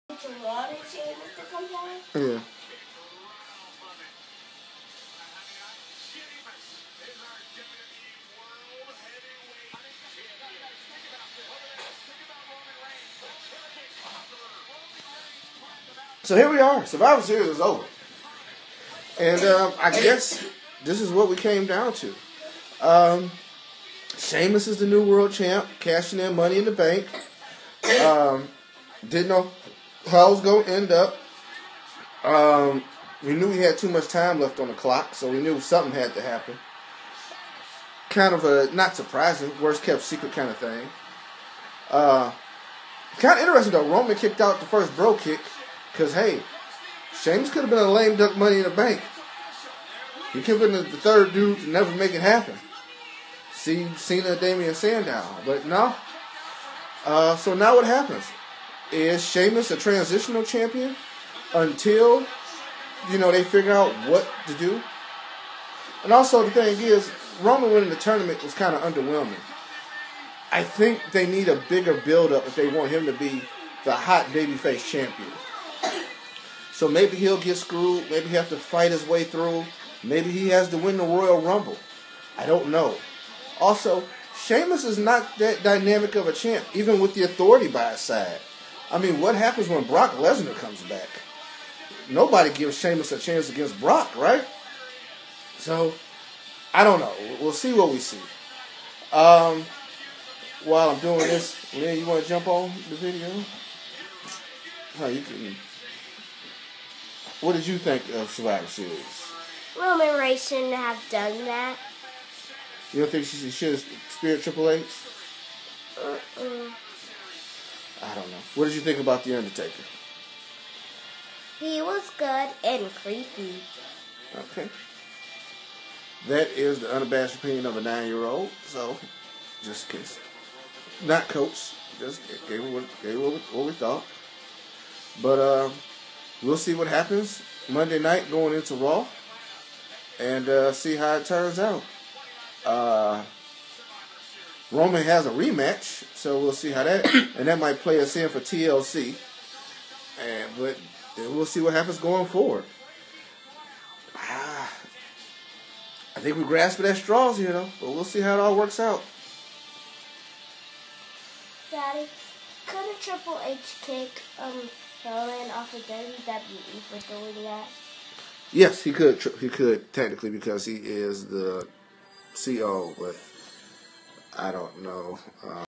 Survivor Series live final
My final thoughts right after the PPV. Be ready for Ring Time Pro Wrestling podcast Wednesday night